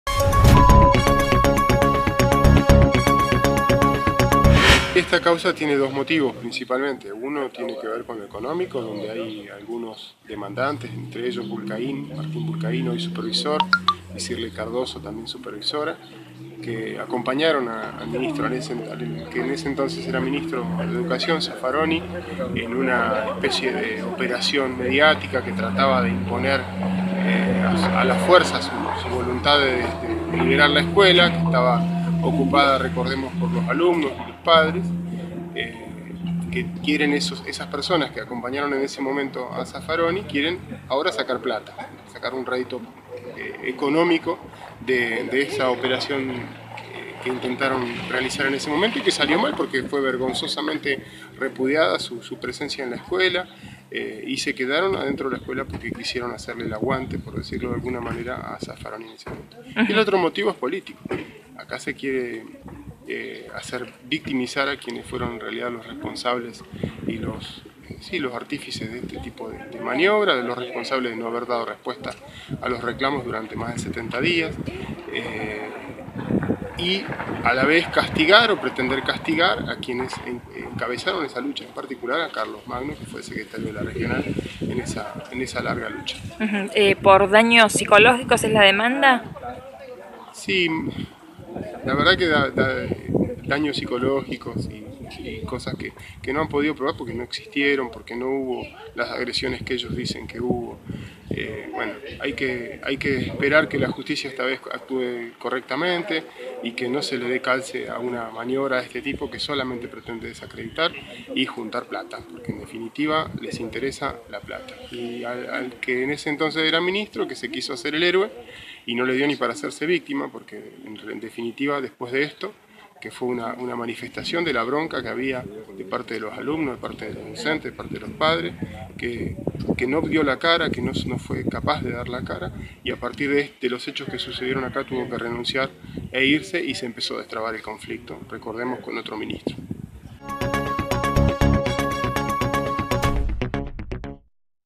TOMA DE UNA ESCUELA: LA JUSTICIA AVANZÓ CON LA INSPECCIÓN OCULAR EN EL HIPOLITO YRIGOYEN
Daniel Murphy / ATECh